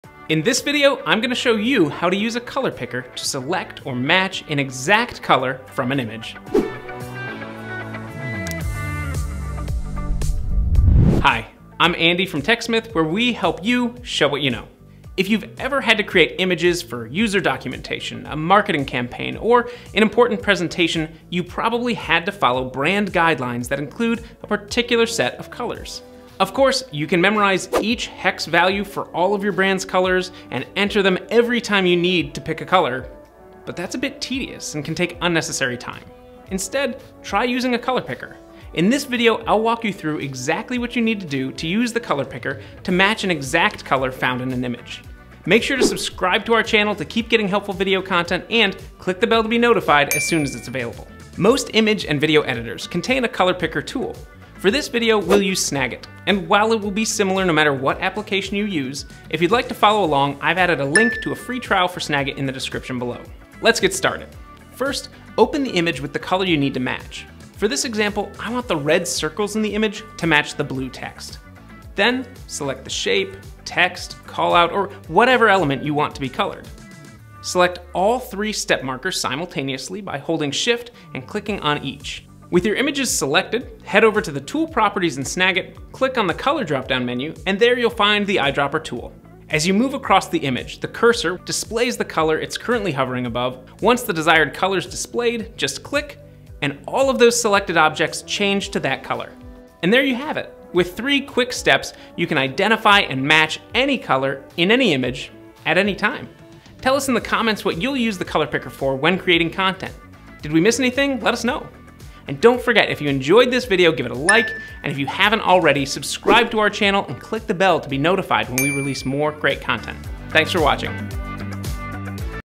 How to Use a Color Picker to Select an Exact Color from an Image
You need to use exact colors when using branding, or even matching another graphic in your image.

A color picker is a tool that allows you to select an exact color of visual elements like text or shapes in a document or graphic.

If you need to create images for a marketing campaign, an important presentation, or user documentation, you probably need to follow brand guidelines that include a particular set of colors.

A color picker allows you to perfectly match brand guidelines or maintain a consistent theme.

Blog post: Use a Color Picker to Select an Exact Color from an Image -

0:00 - Introduction
0:55 - Using Snagit's color picker
1:05 - Step 1 - Open the image you need to match
1:12 - Step 2 - Select the shape, text or annotation you want to change
1:25 - Step 3 - Click the color drop down to find the Eyedropper Tool Color Picker
1:39 - Three quick & easy steps to use a color picker and match colors!